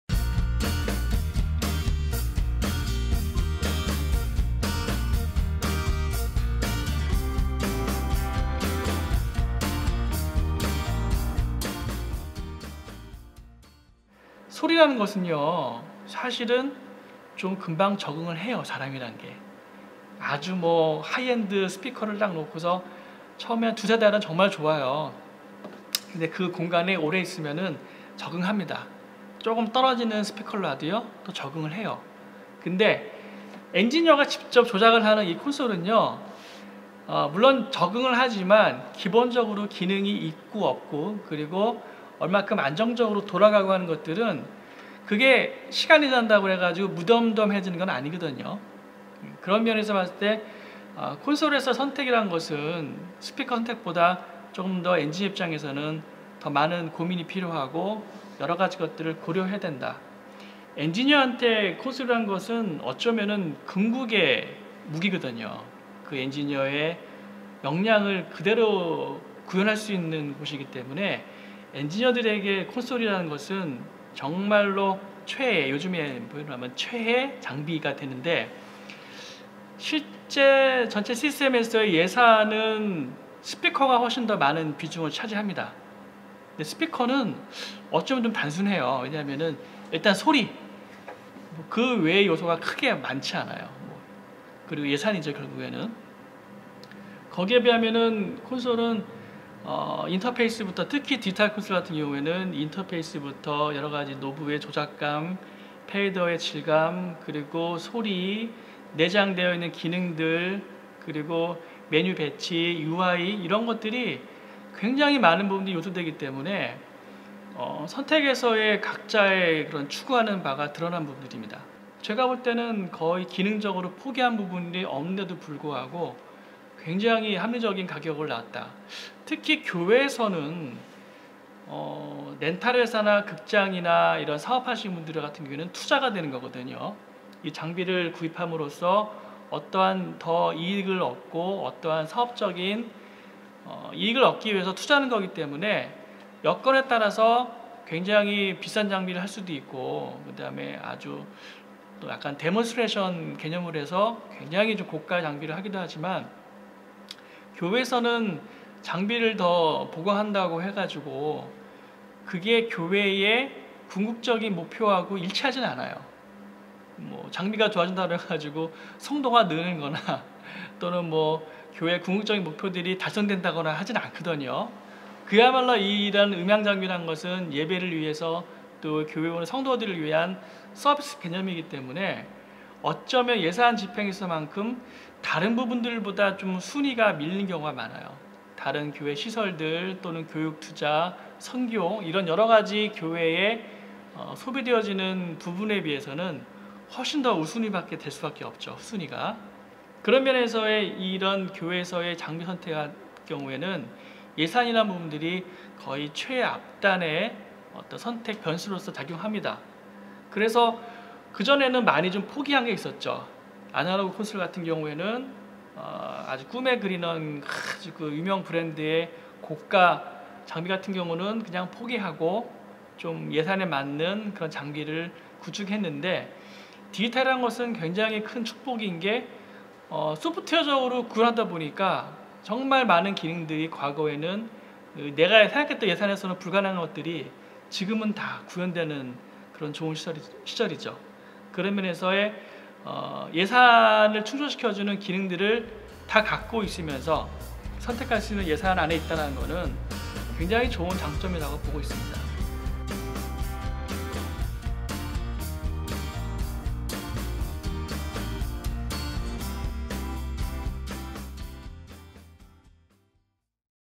[Allen&Heath] 엔지니어가 말하는 합리적인 콘솔 선택 가이드!
Allen&Heath
dLive를 선택한 숭의교회 이한용 실장님 인터뷰!

엔지니어가 직접 조작하는,
엔지니어 입장에서의 합리적인 콘솔 선택 가이드!!

자세한 제품 정보는
에서 확인하실 수 있습니다.